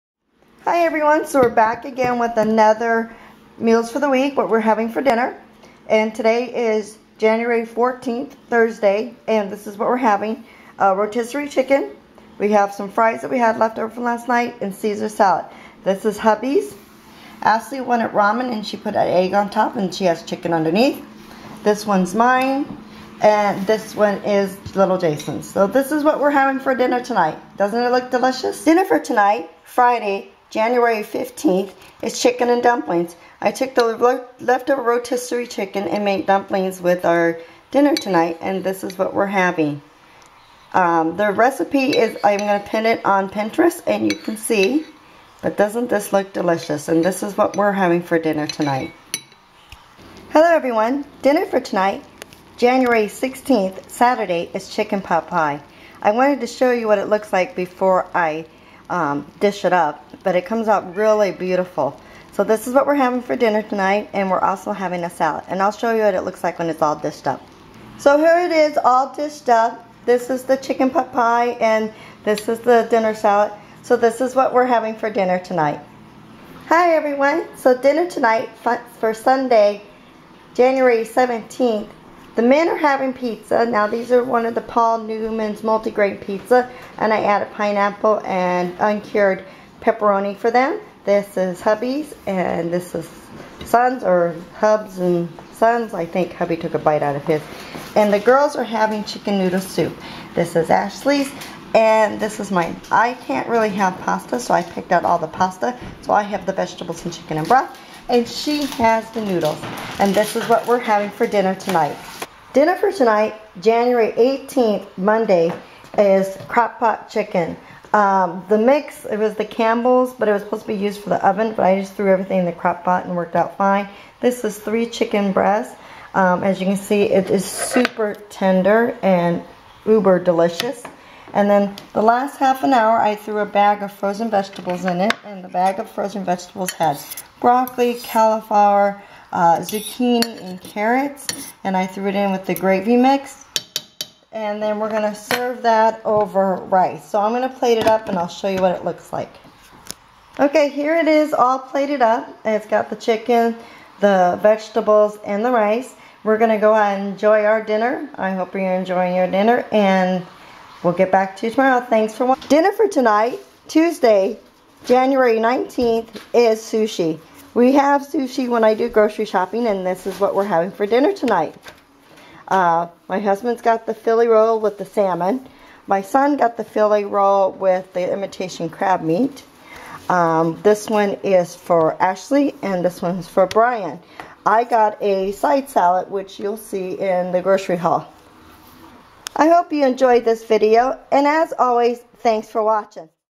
Dinners for the Week of Jan 14th-19th
This is what we ate this week for dinner.